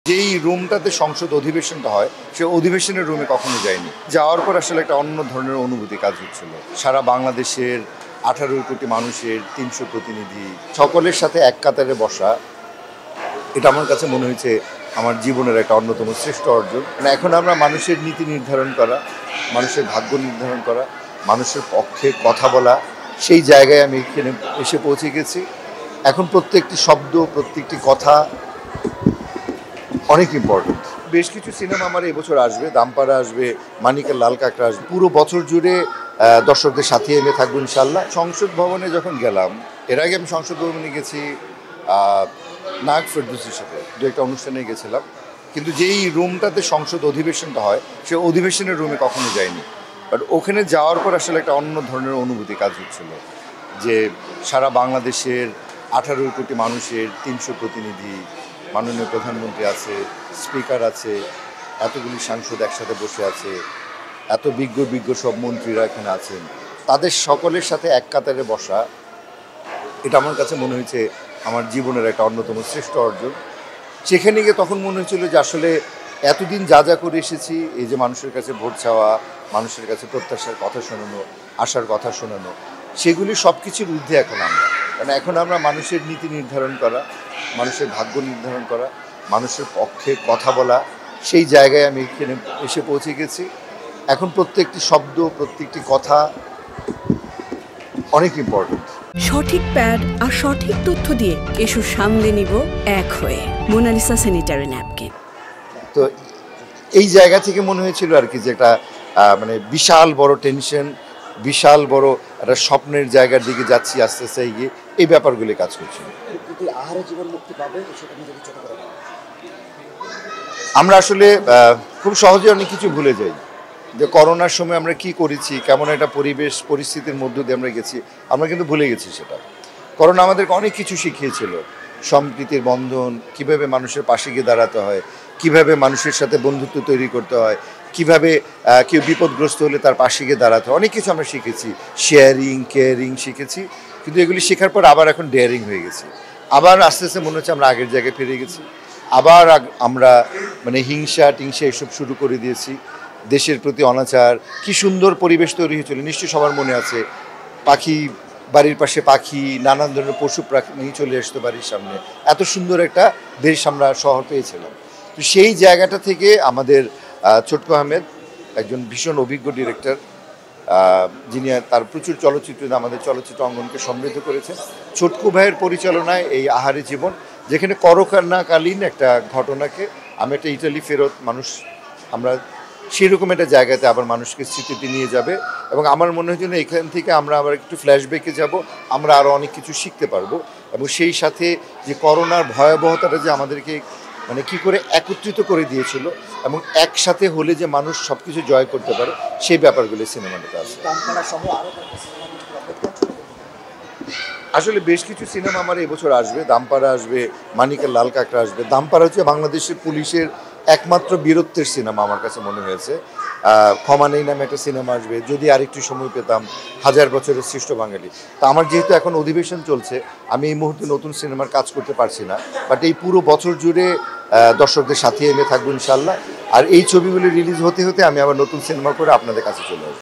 সংসদে ৩০০ সাংসদ, প্রধানমন্ত্রী ও স্পিকারের সামনে বসা আমার জীবনের শ্রেষ্ট অর্জন: ফেরদৌস | News24
NEWS24 Team